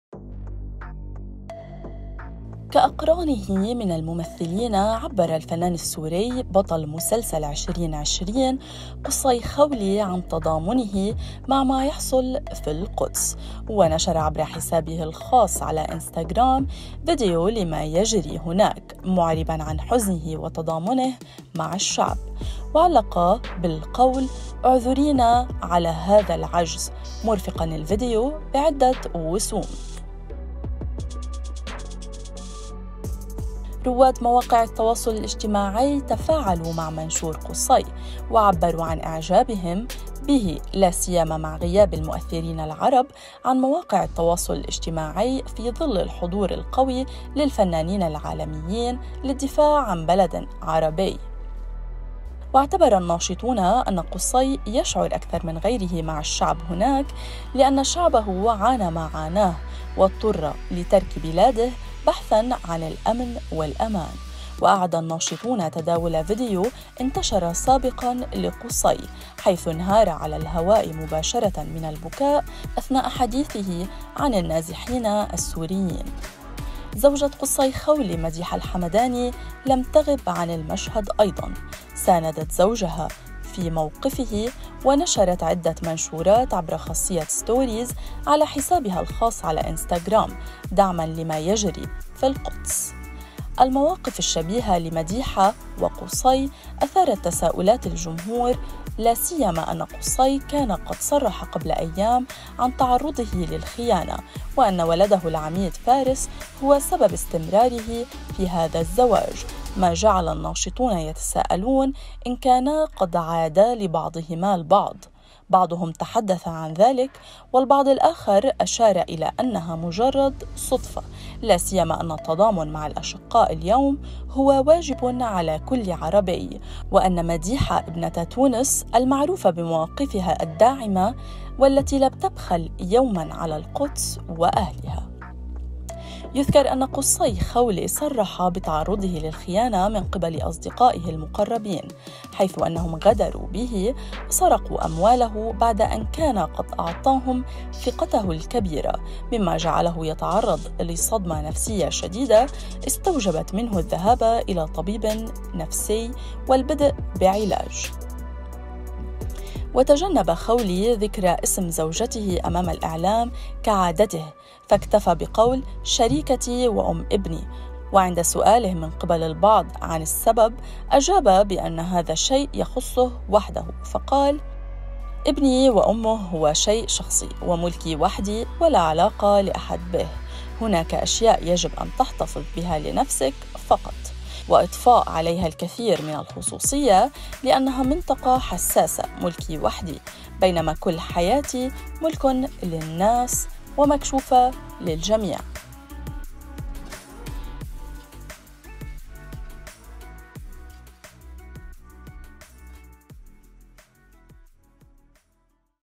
عاجل - قصي خولي ينهار على الهواء مباشرة خلال مقابلة و موقف زوجته صادم !
انهار النجم السوري قصي_خولي على الهواء مباشرة خلال مقابلة تلفزيونية .

و كأقرانه من الممثلين عبر الفنان السوري بطل مسلسل_2020 قصي خولي عن تضامنه مع ما يحصل ونشر عبر حسابه الخاص على انستغرم فيديو لما يجري هناك معربا عن حزنه وتضامنه مع الشعب وعلق على الصورة بالقول اعذرينا على هذا العجز.